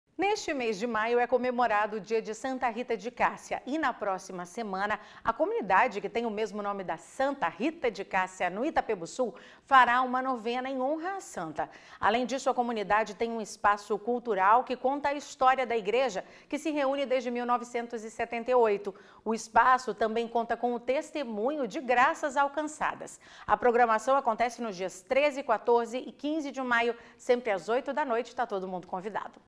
11/05/2024 - NA PRÓXIMA SEMANA A COMUNIDADE SANTA RITA DE CÁSSIA FARÁ UMA NOVENA EM HONRA À SANTA
Neste mês de maio é comemorado o dia de Santa Rita de Cássia, e na próxima semana a comunidade Santa Rita de Cássia, no Itapebussu, fará uma novena em honra à santa. Além disso a comunidade conta com  um espaço cultural que conta a história da igreja que se reúne desde 1978.